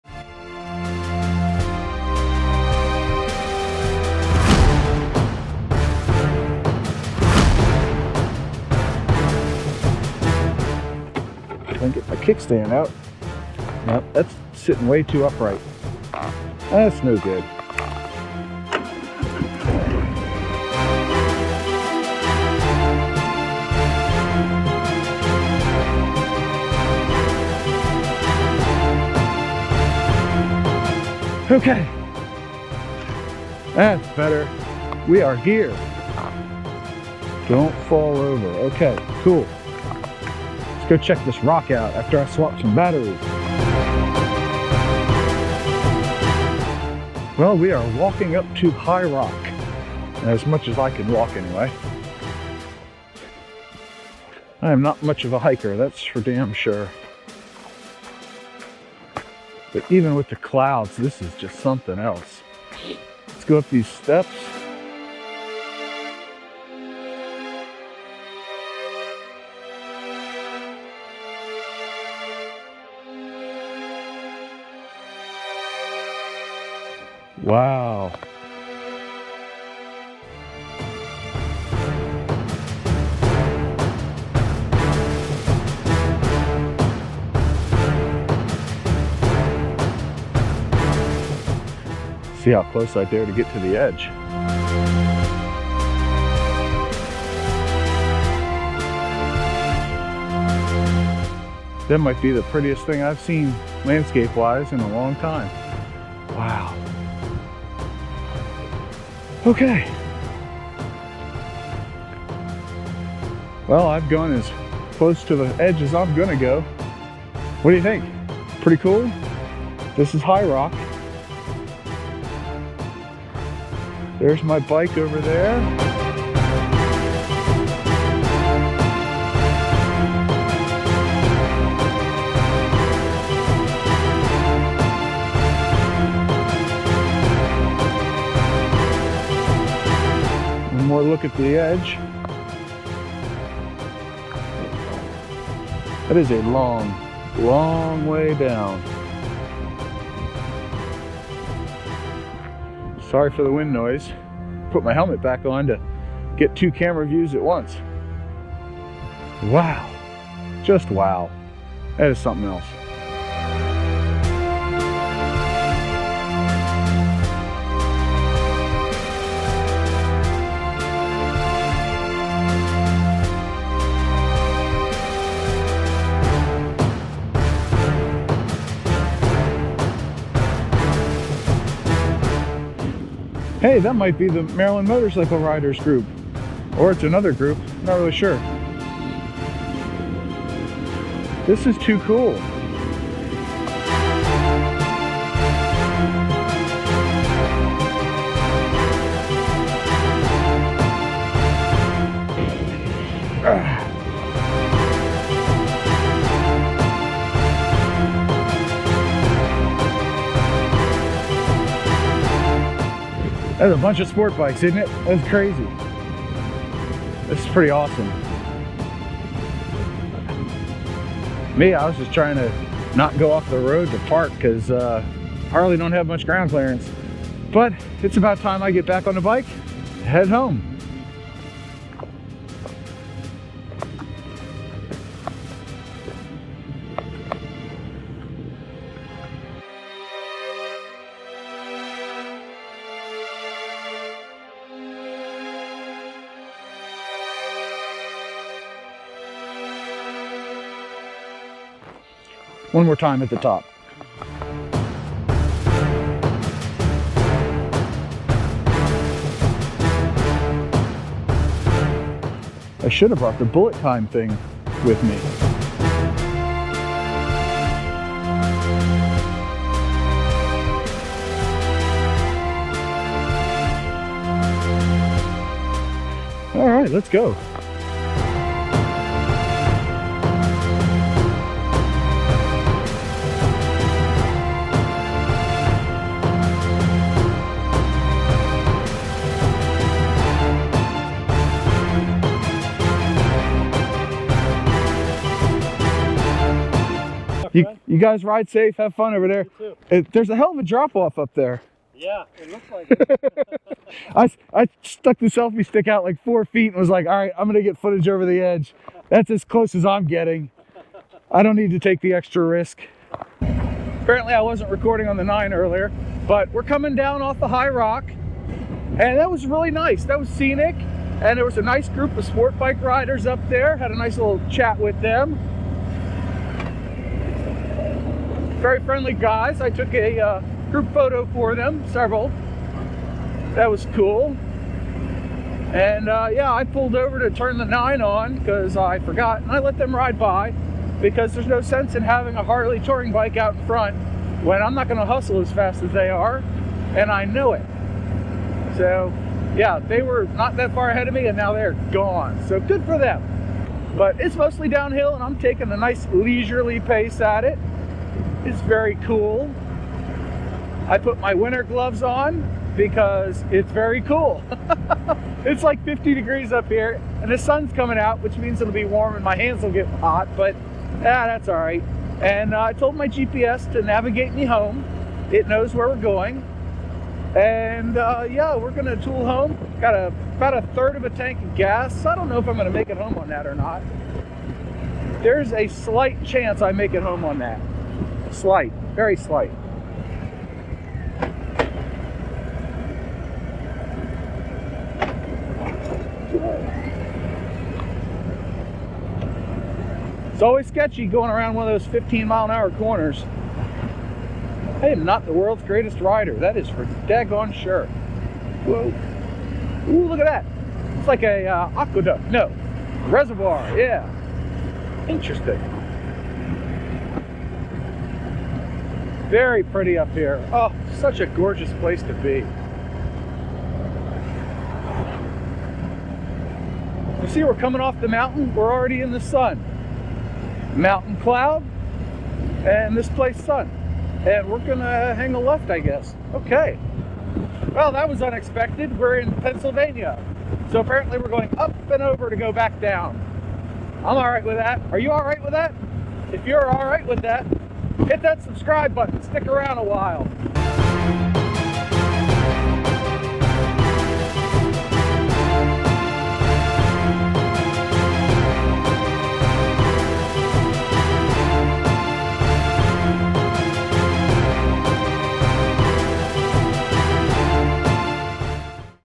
High Rock // Part 3 // At High Rock
As you saw in Part 2 of my High Rock Ride, I'm HERE!  In this part, I walk around, get some NICE shots of High Rock, and begin the journey home.  Enjoy!

GoPro Hero 9
GoPro Hero 9 Media Mod
GoPro Hero 8
GoPro Hero 8 Media Mod
RickRak Street Glide Fairing Mount